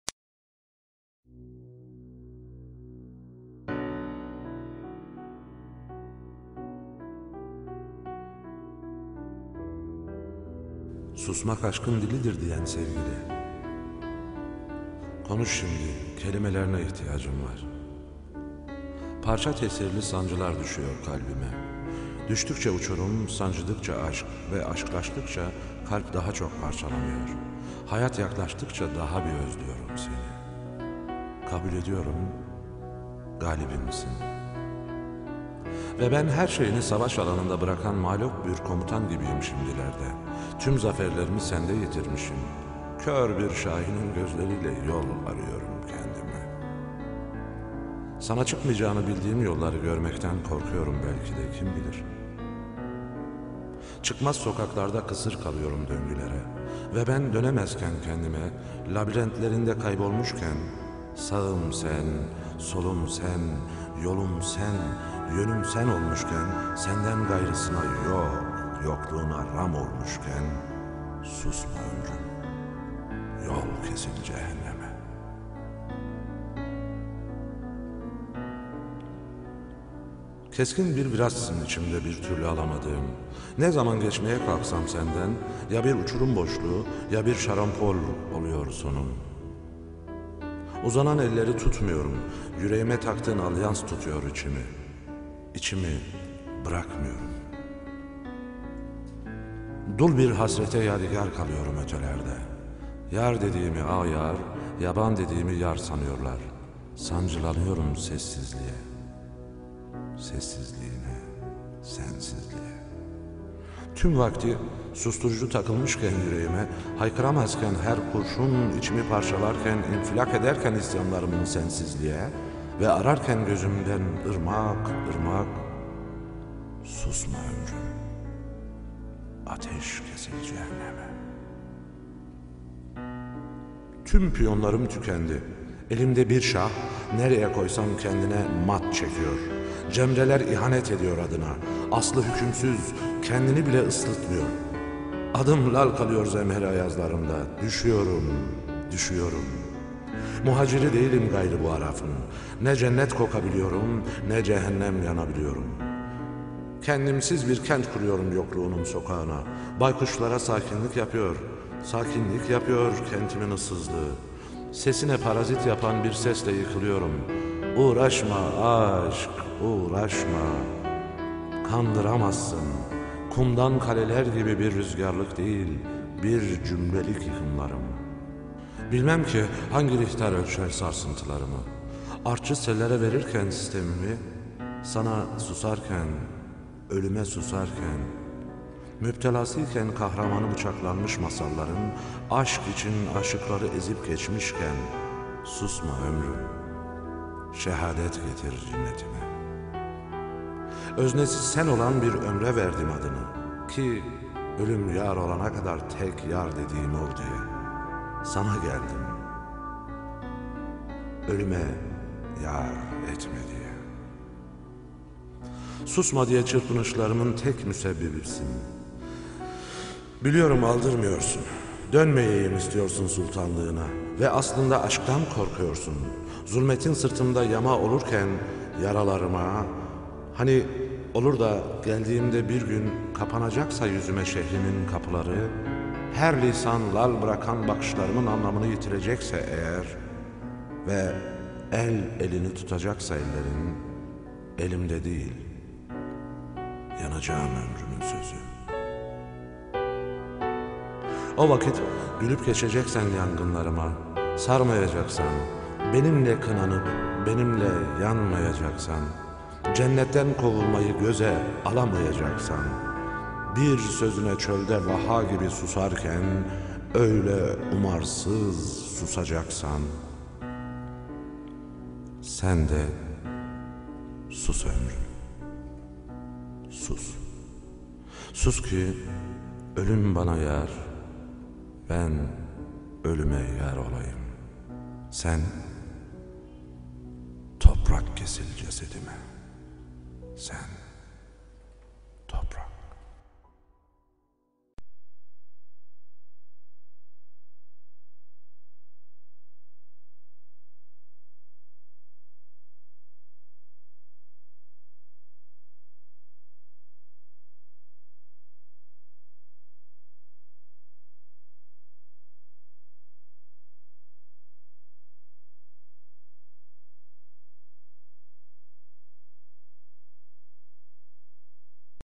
'' Galibimsin '' - - -Hüseyin KARAKUŞ- - -
susmak aşkın dilidir diyen sevgili konuş şimdi kelimelerine ihtiyacım var...

Parça tesirli sancılar düşüyor kalbime..

Düştükçe uçurum, sancıdıkça aşk...Ve aşklaştıkça kalp daha çok parçalanıyor, hayat yaklaştıkça daha bir özlüyorum.

Kabul ediyorum, galibimsin!..

Ve ben her şeyini savaş alanında bırakan mağlup bir komutan gibiyim şimdilerde...

Tüm zaferlerimi sende yitirmişim...Kör bir şahinin gözleriyle yol arıyorum kendime.Sana çıkmayacağını bildiğim yolları görmekten korkuyorum belki de kim bilir?

Çıkmaz sokaklarda kısır kalıyorum döngülere...

Ve ben dönemezken kendime, labirentlerinde kaybolmuşken,

Sağım sen, solum sen, yolum sen, yönüm sen olmuşken,

Senden gayrısına yok, yokluğuna ram olmuşken,

Susma ömrüm!..

Yol kesil cehenneme...

Keskin bir virajsın içimde bir türlü alamadığım...

Ne zaman geçmeye kalksam senden;

Ya bir uçurum boşluğu, ya bir şarampol oluyor sonum...

Uzanan elleri tutmuyorum...Yüreğime taktığın alyans tutuyor içimi,

İçimi bırakmıyorum...

Dul bir hasrete yadigar kalıyorum ötelerde.

Yar dediğimi ağyar, yaban dediğimi yar sanıyorlar...

Sancılanıyorum sessizliğine

Tüm vakti;

Susturucu takılmışken yüreğime,

Haykıramazken, her kurşun içimi parçalarken,

İnfilak ederken isyanlarım sensizliğe,

Ve akarken gözümden ırmak ırmak,

Susma ömrüm!..

Ateş kesil cehenneme...

Tüm piyonlarım tükendi...

Elimde bir şah,

Nereye koysam kendine mat çekiyor...

Cemreler ihanet ediyor adına,

Aslı hükümsüz...Kendini bile ısıtmıyor...

Adım lal kalıyor zemheri ayazlarına,

D-üşüyorum!..

Muhaciri değilim gayrı bu Arafın...

Ne cennet kokabiliyorum, ne cehennem yanabiliyorum...

Kendimsiz bir kent kuruyorum yokluğunun sokağına...

Baykuşlara sakinlik yapıyor kentimin ıssızlığı,

Sesine parazit yapan bir sesle yıkılıyorum

Uğraşma aşk!..

Kal(n)dıramazsın;

Kumdan kaleler gibi bir rüzgarlık değil, bir cümlelik yıkımlarım...

Bilmem ki hangi rihter ölçer sarsıntılarımı,

Artçı sellere verirken sitemimi,

Sana ''sus''arken,

Ölüme ''sus''arken,

Müptelasıyken kahramanı bıçaklanmış masalların

Aşk için aşıkları ezip geçmişken,

Susma ömrüm!..

Şehadet getir cinnetime...

Öznesi sen olan bir ömre verdim adını,

Ki ölüm yar olana kadar tek yar dediğim ol diye!..

Sana geldim, ölüme yar etme diye.

Susma diye çırpınışlarımın tek müsebbibisin...

Biliyorum aldırmıyorsun,

Dönmeyeyim istiyorsun sultanlığına.

Ve aslında AŞK'tan korkuyorsun

Zulmetin sırtımda yama olurken yar'alarıma!..

Hani olur da geldiğimde bir gün;

Kapanacaksa yüzüme şehrinin kapıları,

Her lisanı lal bırakan bakışlarım anlamını yitirecekse eğer

Ve el elini tutacaksa ellerin,

Elimde değil, yanacağım!..

O vakit gülüp geçeceksen yangınlarıma,

Sarmayacaksan,

Benimle kınanıp, benimle yanmayacaksan,

Cennetten kovulmayı göze alamayacaksan,

Bir sözüne çölde vaha gibi susarken

Öyle umarsız susacaksan;

Sen de sus ömrüm!..

Sus!..

Sus ki; ölüm bana yar, ben ölüme YAR olayım...

Sen toprak kesil cesedime!..

ŞİİR : ALINTI (KYNK : SD) )
YORUM : HÜSEYİN KARAKUŞ
VİDEO :SD (suyum dersimli)